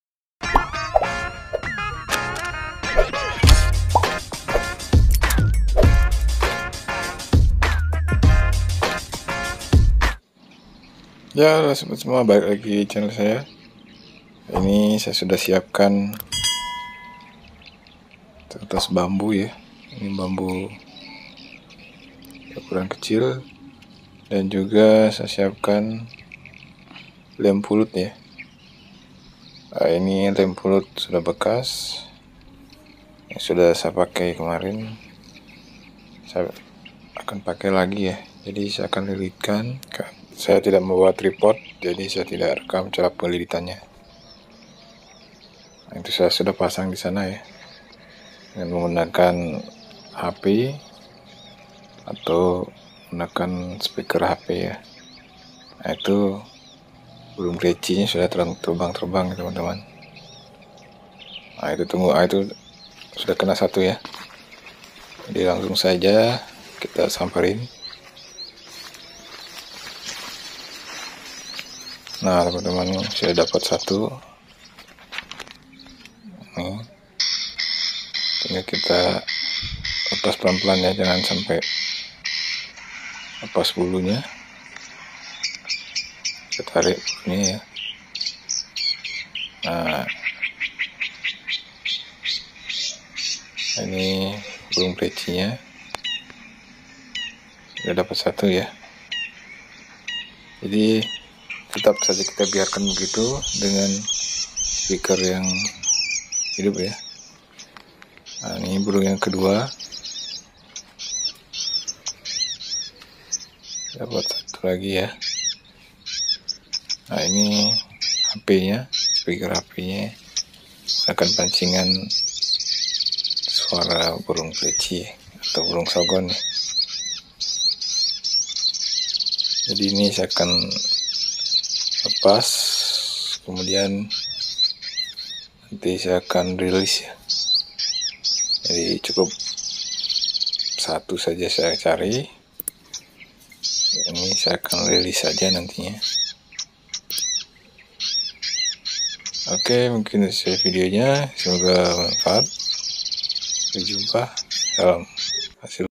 Cara Pikat Burung dengan Hanya Bermodalkan HP dan Pulut gampang dan dapat banyak
Cara Pikat Burung dengan Hanya Bermodalkan HP dan Pulut gampang dan dapat banyak gaes.